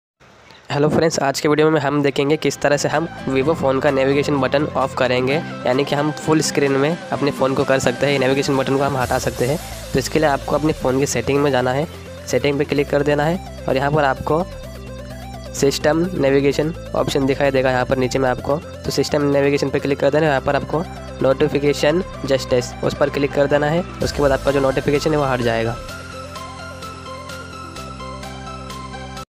how to hide navingastin button smartphone | vivo mobile new secret setting
hello guys is video mein main aapko batane wala hun vivo phone Mai navigation button Kaise hide Karte Hain

@baby 4you